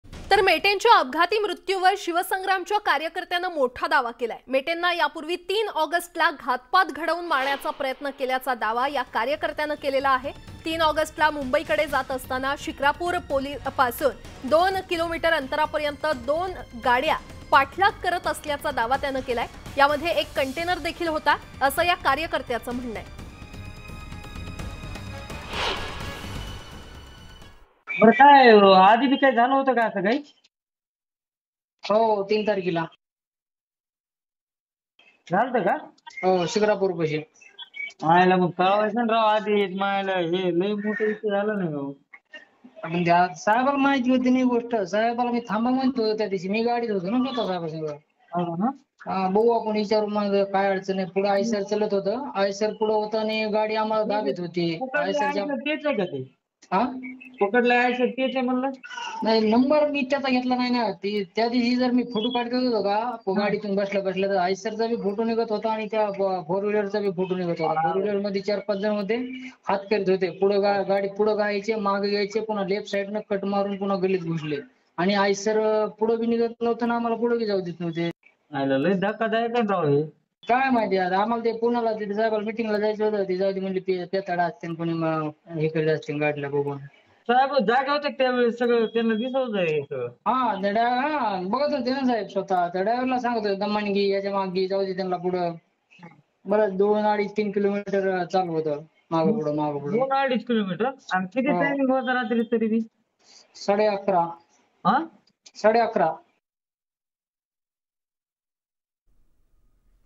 Vinayak Mete Car Accident : मेटेंना 3 ऑगस्टलाही मारण्याचा प्रयत्न, मेटेंचा मृत्यू घातपात की अपघात?
President of the Shiv Sangram Sanghatana Vinayak Mete, had met with an accident on the Mumbai-Pune Expressway. Mete has died in the accident. The driver of the truck that was hit by Mete's car has finally been taken into custody.

शिवसंग्राम संघटनेचे अध्यक्ष विनायक मेटे (Vinayak Mete Death)  यांचा मुंबई पुणे एक्स्प्रेस वेवर अपघात झाला होता. या अपघातात मेटे यांचं निधन झालं आहे. ज्या ट्रकला मेटेंची गाडी धडकली होती अखेरीस त्या ट्रकचालकाला ताब्यात घेतले आहे.

n18oc_news